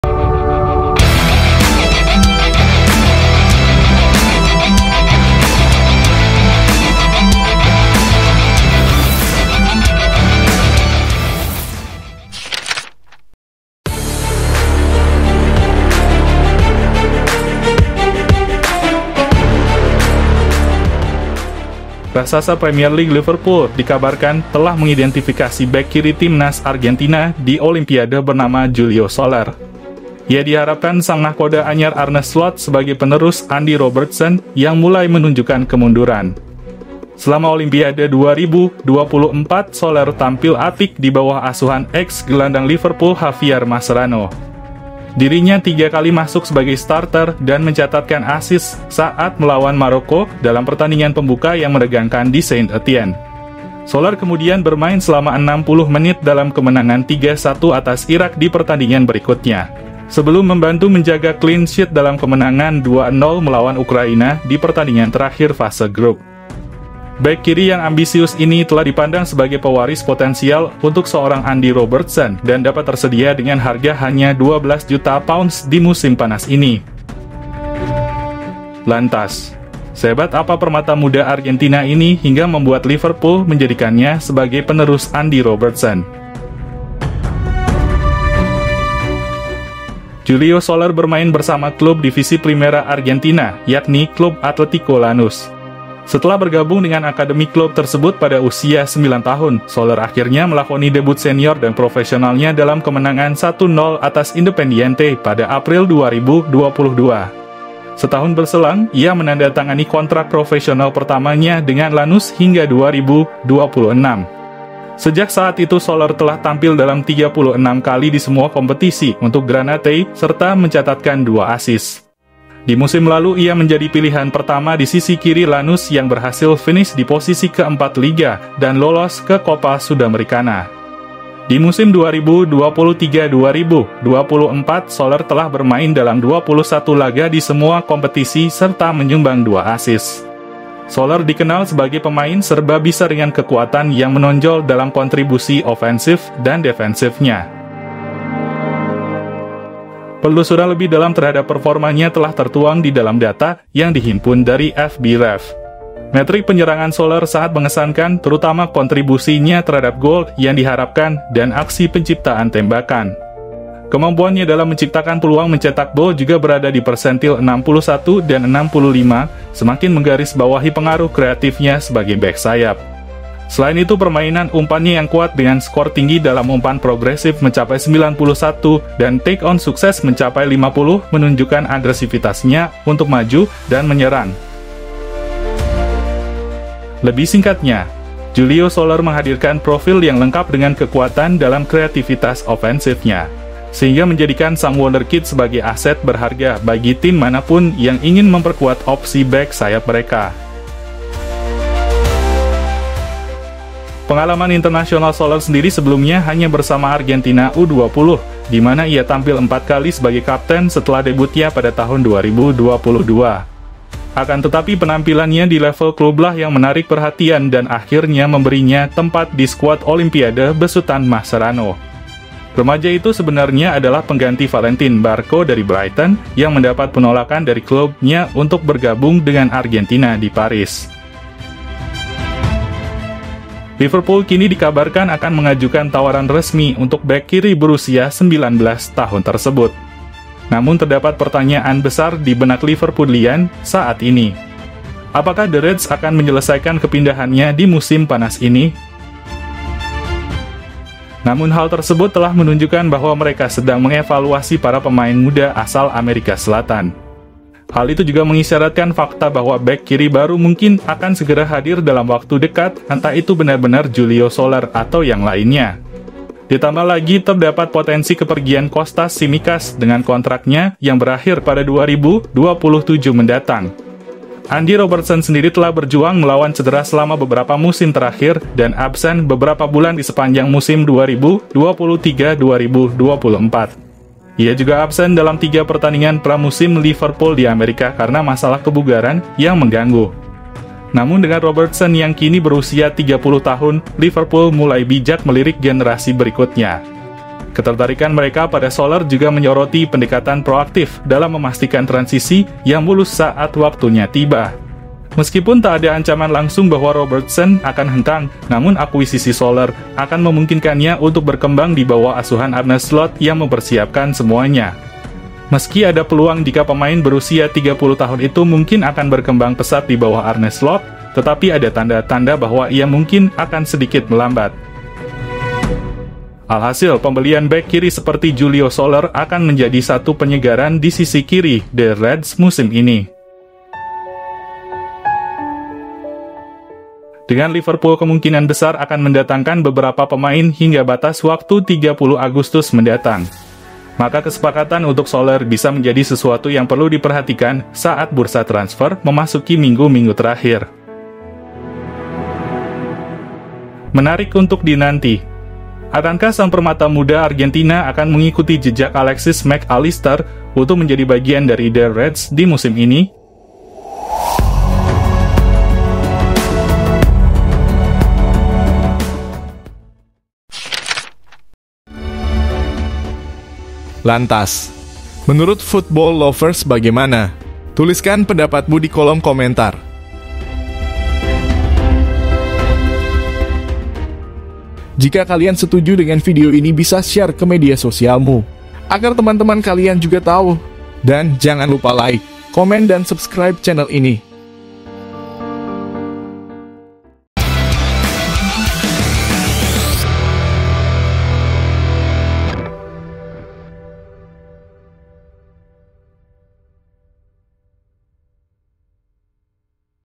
DARI PERMATA BARU ARGENTINA JADI PENERUS ANDY ROBERTSON DI LIVERPOOL, APA HEBATNYA JULIO SOLER?
RAKSASA PREMIER LEAGUE LIVERPOOL DIKABARKAN TELAH MENGIDENTIFIKASI BEK KIRI TIMNAS ARGENTINA DI OLIMPIADE BERNAMA JULIO SOLER. IA DIHARAPKANN SANG NAHKODA ANYAR THE REDS ARNE SLOT SEBAGAI PENERUS ANDY ROBERTSON YANG MULAI MENUNJUKAN KEMUNDURAN. SELAMA OLIMPIADE 2024, SOLER TAMPIL APIK DI BAWAH ASUHAN EKS GELANDANG LIVERPOOL JAVIER MASCHERANO. LANTAS. SEHEBAT APA PERMATA MUDA ARGENTINA INI, HINGGA MEMBUAT LIVERPOOL MENJADIKANNYA SEBAGAI PENERUS ANDY ROBERTSON. SIMAK VIDEO LENGKAPNYA !